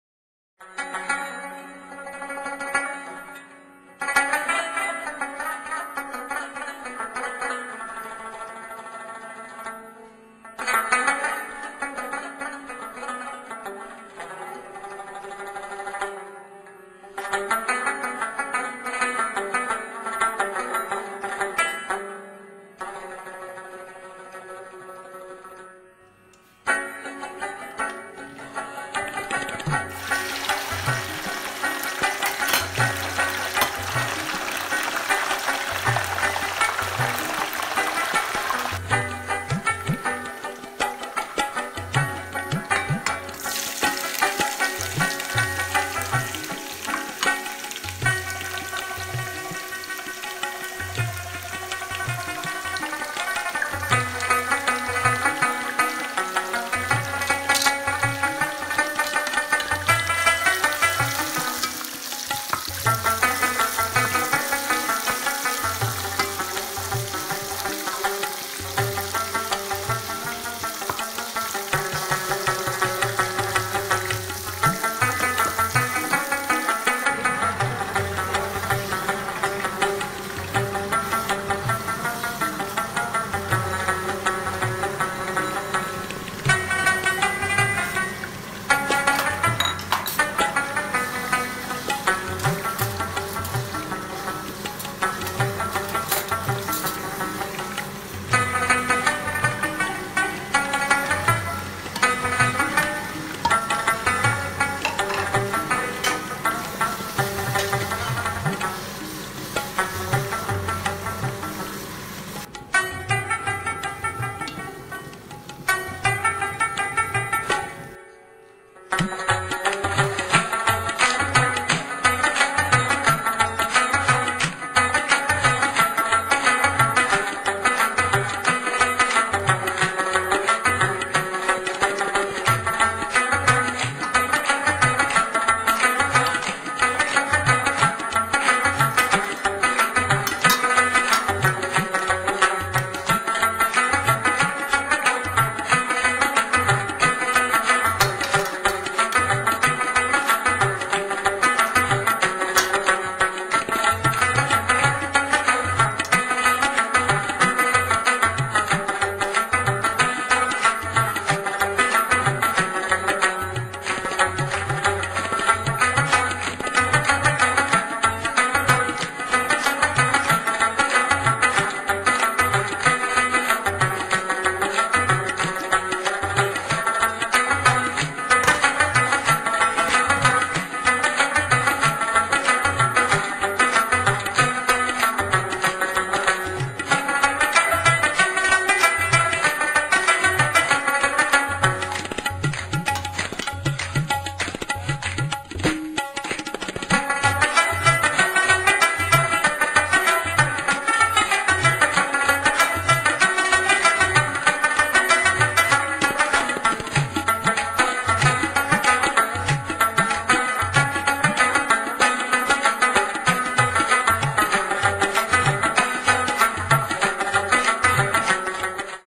How to cook Okra/ طرز تهيه باميه مزاري
How to cook Okra طرزتهيه باميه مزاري
مواد لازمه
يك كيلو باميه   1  kg okra
 روغن به قدر ضرورت    amount of necessary oil
سه عدد پياز.  Orion
يك قوطي كنسل رومي   One can of tomato
مرچ تازه   fresh chilli
يك قاشق غذا خوري گشنيز خشك   One spoon of coriander seeds
نيم قاشق چاي خوري مرچ سياه.  Half a spoon of black pepper
يك قاشق غذا خوري مرچ سرخ  want spawn red chilli
نمك يك قاشق غذا خوري  Half a spoon of salt
سير.  Garlic
رومي   Tomato